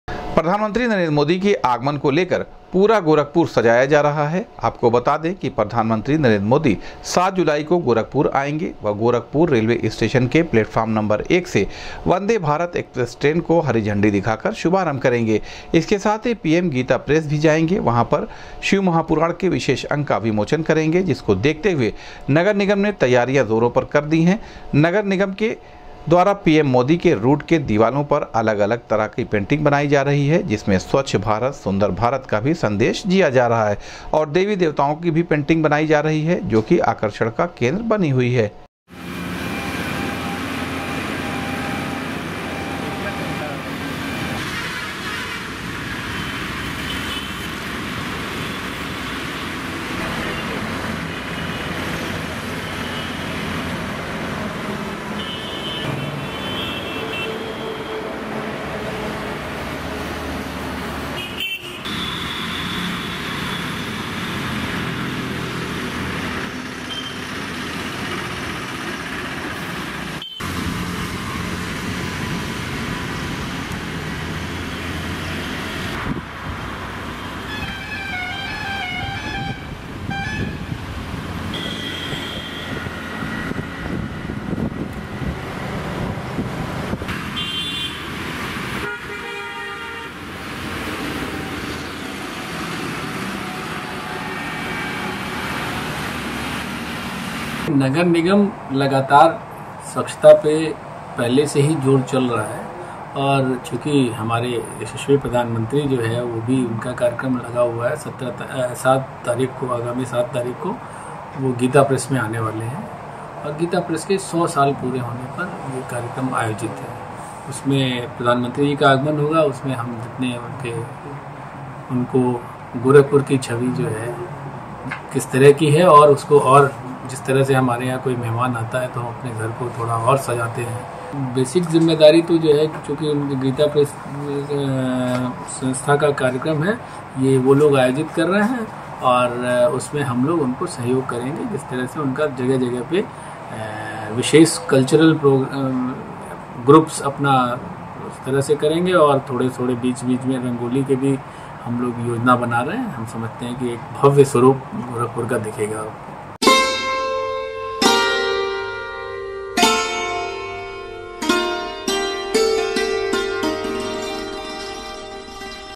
प्रधानमंत्री नरेंद्र मोदी के आगमन को लेकर नगर निगम के द्वारा पूरे गोरखपुर को जा रहा है सजाया
प्रधानमंत्री नरेंद्र मोदी के आगमन को लेकर नगर निगम के द्वारा पूरे गोरखपुर को जा रहा है सजाया,पीएम के रूट के दिवालो पर बनाया जा रहा है अलग-अलग देवी देवताओं की कलाकृतियां, जो बना है आकर्षण का केंद्र